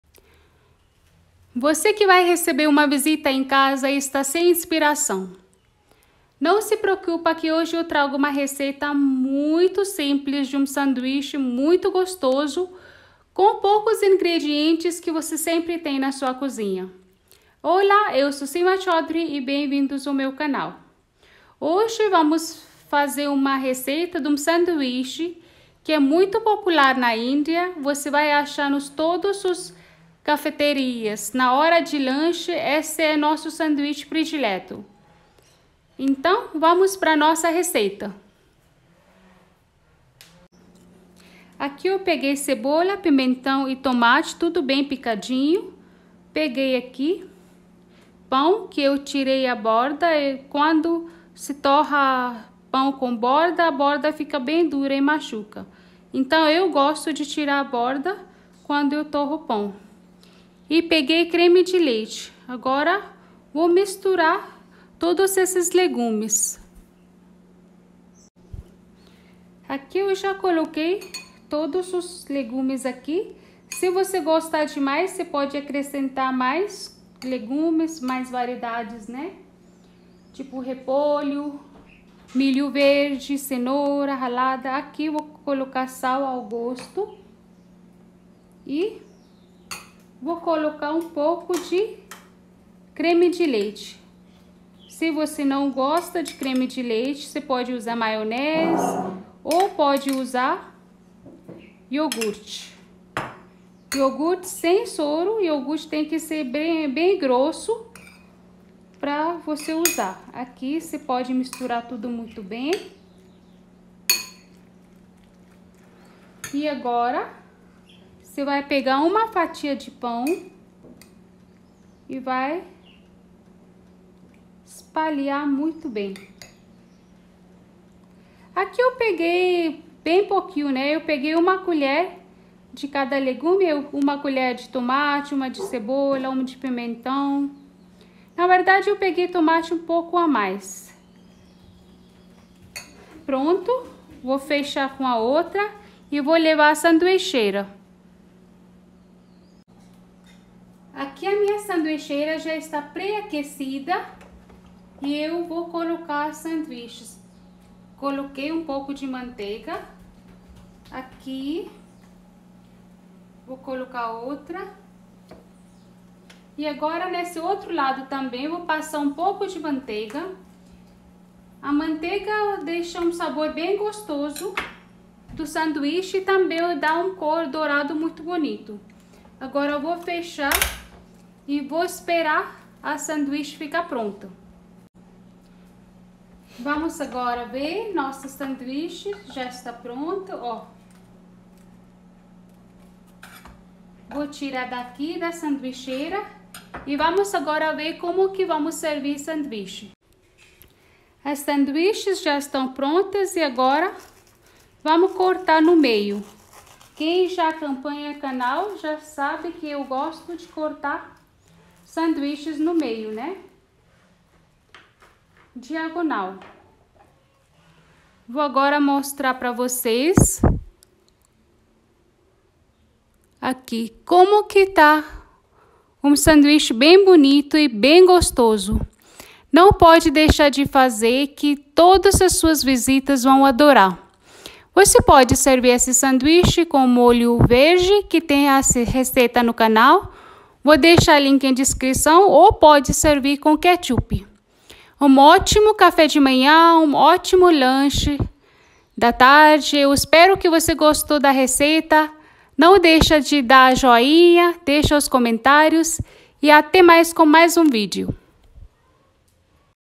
Sanduíche super gostoso e simples| LANCHE RÁPIDO
Link para green Chutney

Receitas de sanduíches

Ingredientes

Pão
Tomate picado
Cebola picada
Pimentão picada
Sal ao gosto
Creme de leite
Manteiga

Modo de preparar

Coloca cebola, tomate, pimentão num pote e adicione sal e creme de leite. Misture bem e aplica sobre um pão, fecha com outro pão.
Passa manteiga dos dois lados e leva ao sanduícheira.
Quando ficar pronto tira e corta em diagonal.
Pode servir com green Chutney ou ketchup.